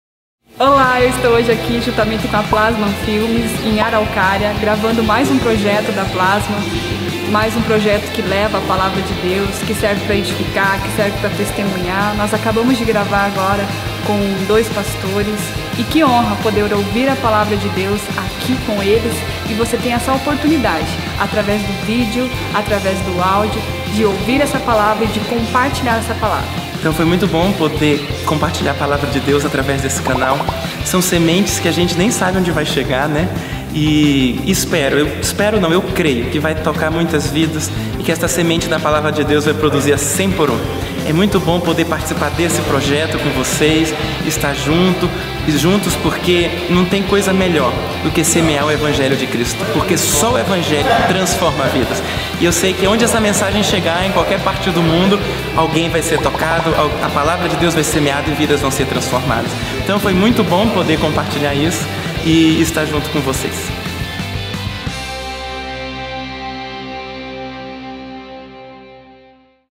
FOR YOU - Karina Cunha e Danilo Mouta
Um breve comentário de Karina Cunha e Danilo Mouta como foi participar das gravações do vídeo FOR YOU da Plassmann Films.
A brief commentary by Karina Cunha and Danilo Mouta as it was to participate in the recordings of the video FOR YOU of Plassmann Films.